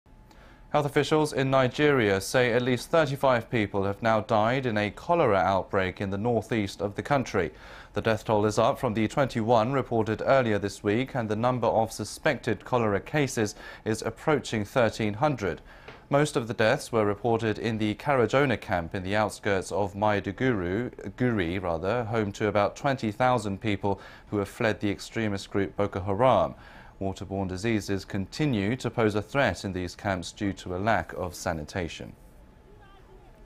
Nigeria cholera outbreak kills 35
나이지리아 북동부 콜레라 확산...35명 사망

  Health officials in Nigeria say at least 35 people have now died in a cholera outbreak in the northeast of the country.
  The death toll is up from the 21 reported earlier this week and the number of suspected cholera cases is approaching 13-hundred.
Most of the deaths were reported in the Karajona camp in the outskirts of Maiduguri, home to about 20-thousand people who have fled the extremist group Boko Haram.
Water-borne diseases continue to pose a threat in these camps due to a lack of sanitation.